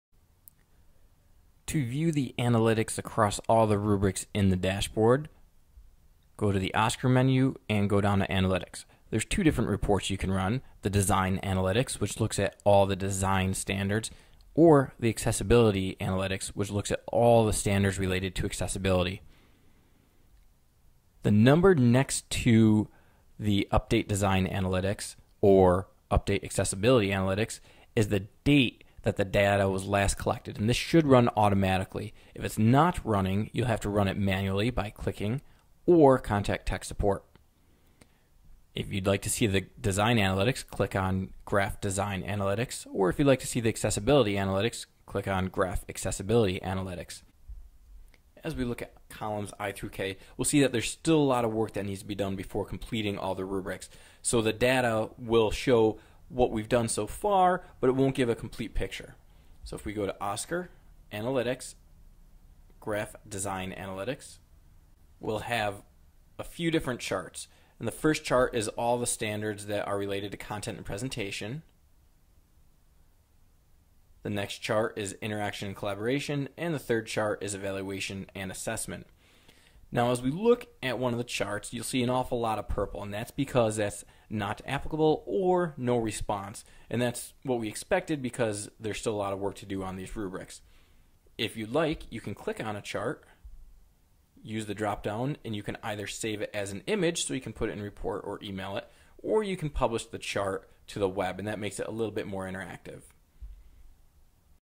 OSCQR Dashboard - Analytics - Viewing
How to view the analytics (both the design analytics and the accessibility analytics) from the OSCQR Dashboard